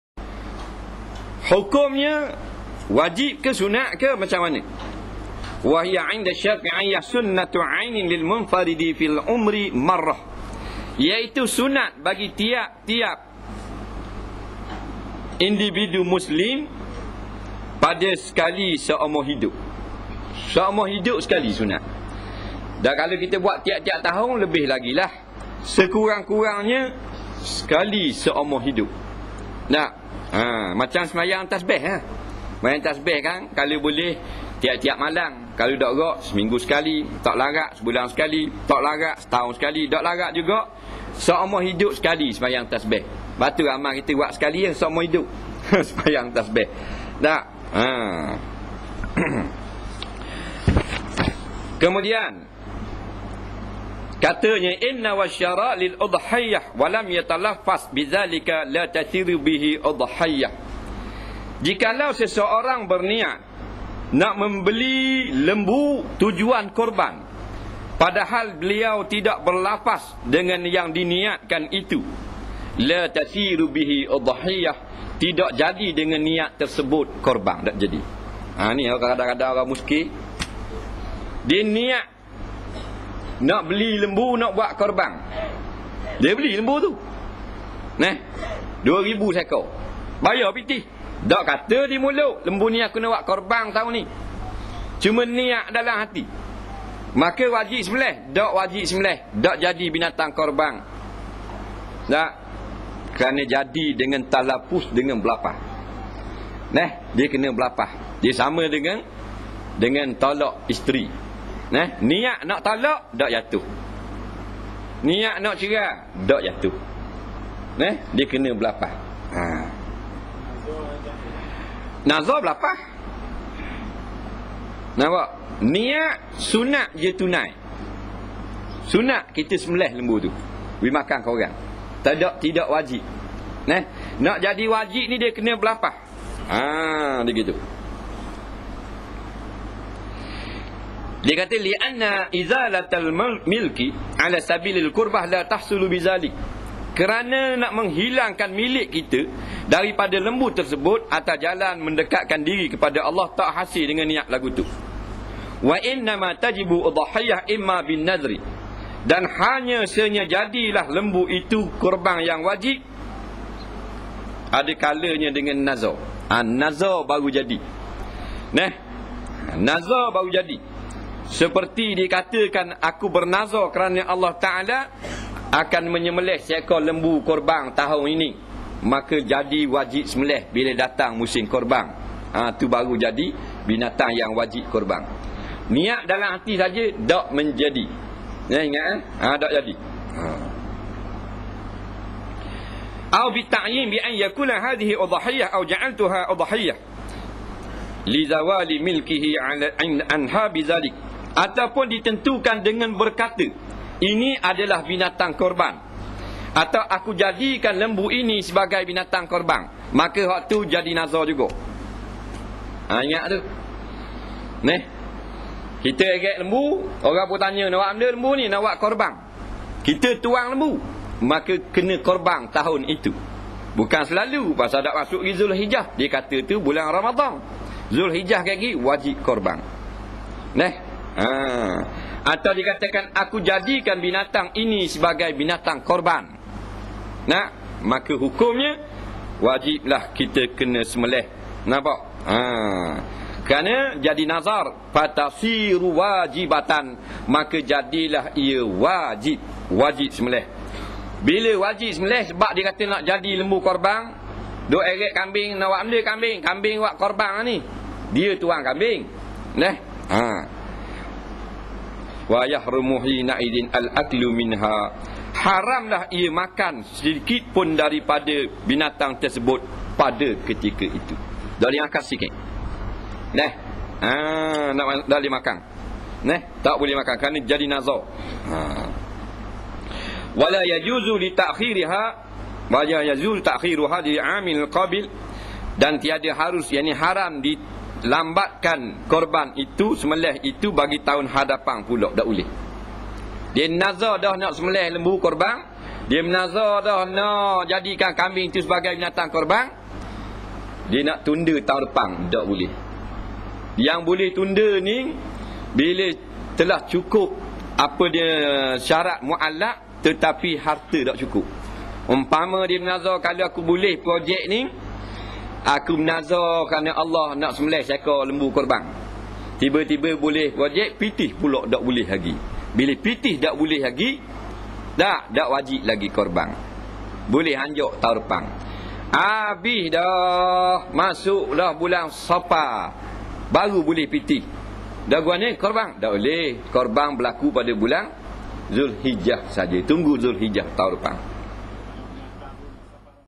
Ustaz Azhar (Ladang Selasa 18.12.7)
Kuliyah Maghrib Mingguan Ustaz Azhar Idrus Di Surau Ladang pada 18 Disember 2007 - Selasa